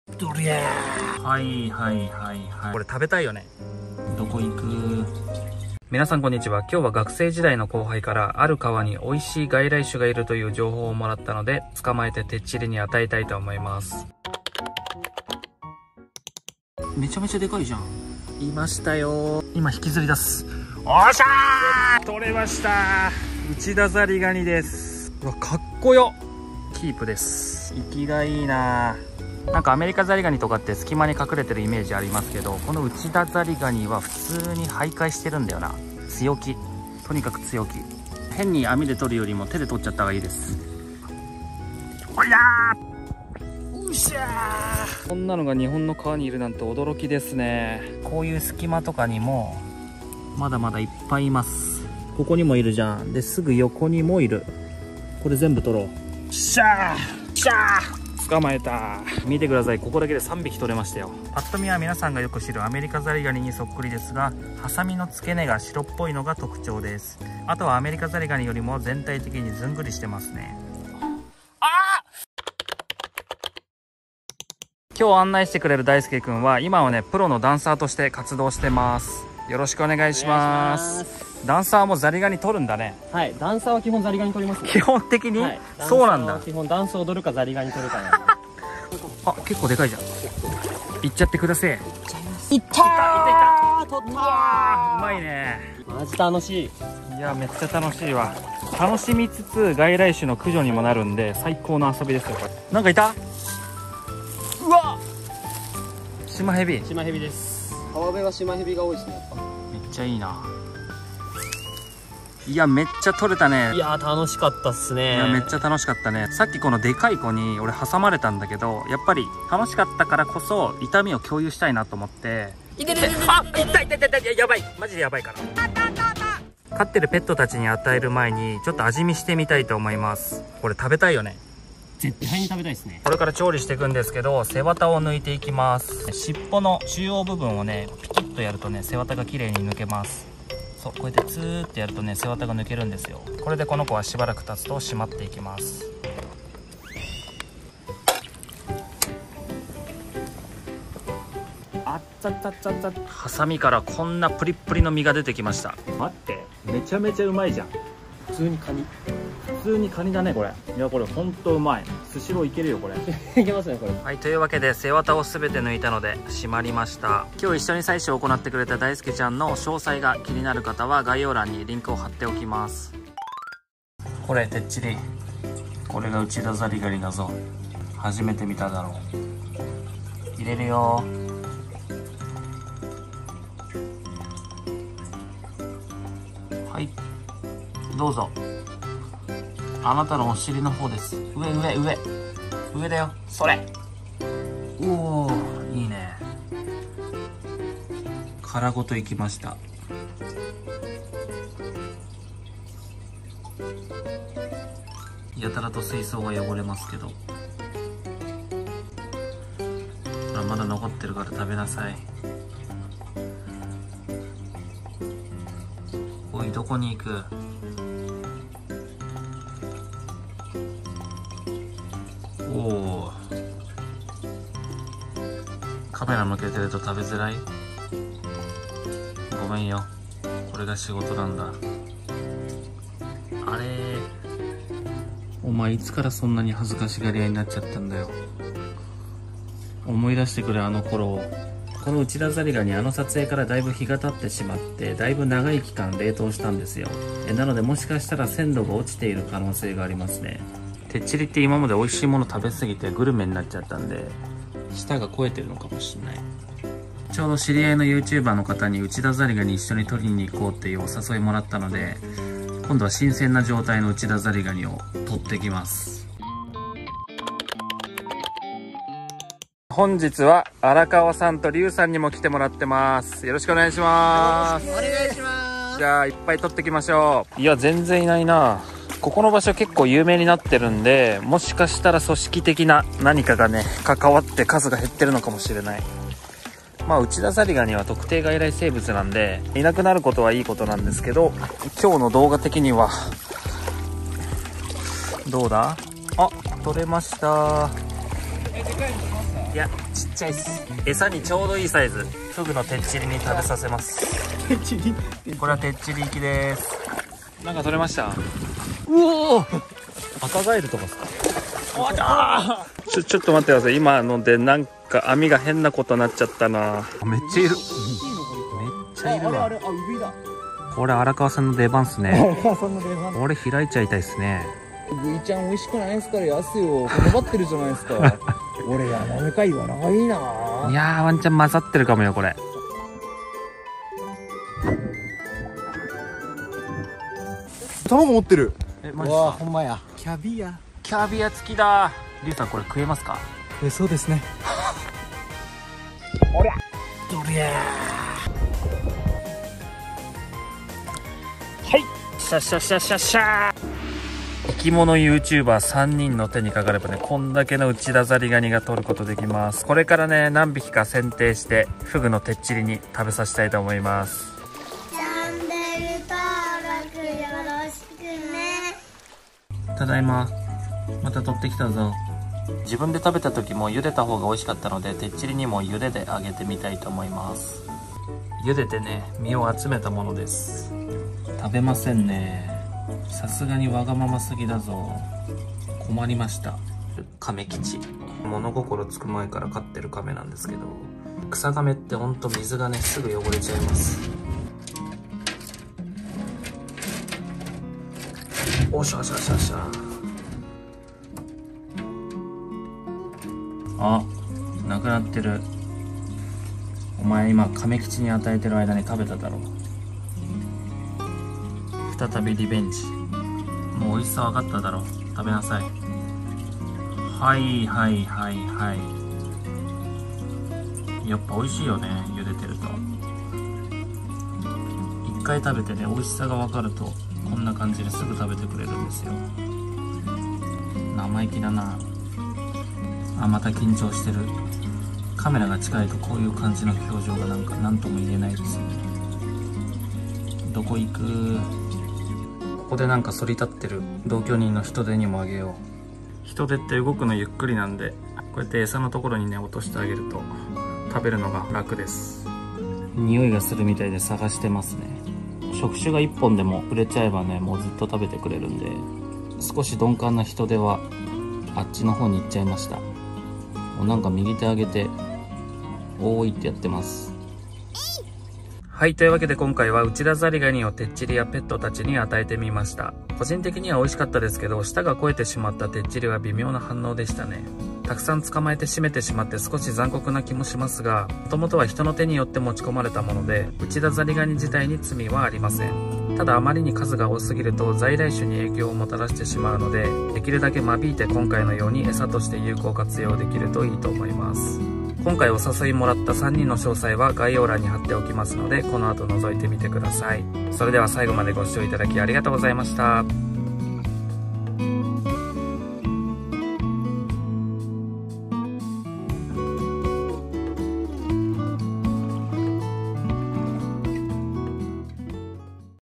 川に発生した外来ザリガニを大量に獲ってフグに与えてみると...！！
コラボありがとうございました！！✨

■人気のカニちゃんの動画まとめ（上から時系列通りに並んでます）

◽️エマスちゃんねる主 もりぽこSNS関連